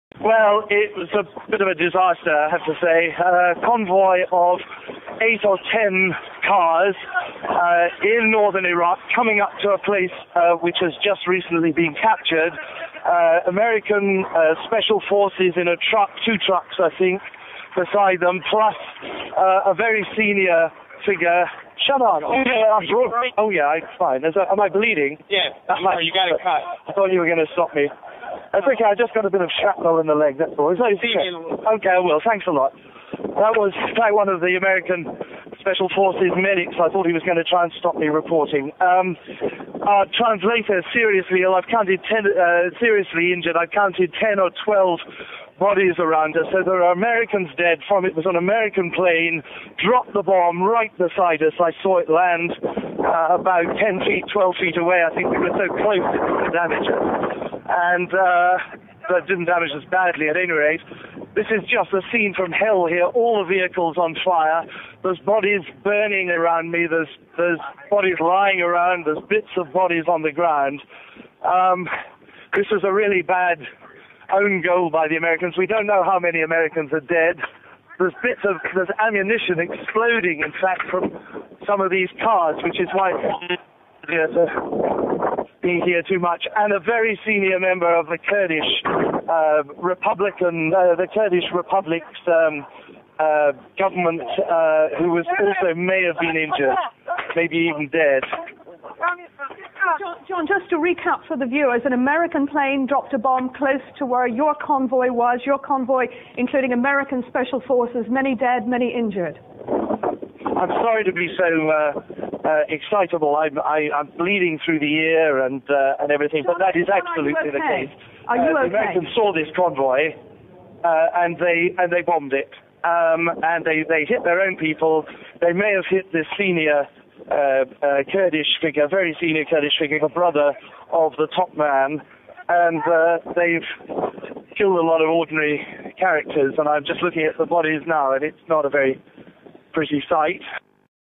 John Simpson phones in a friendly fire bomb report.mov
6th April 2003 - The BBC's world affairs editor John Simpson was accompanying a convoy of US special forces and Kurdish fighters when it came under attack from an American warplane.

At least 10 people were killed, including a Kurdish translator working with the BBC team, Kamaran Abdurazaq Muhamed

Taken from a very low quality source: 34 kbps stream!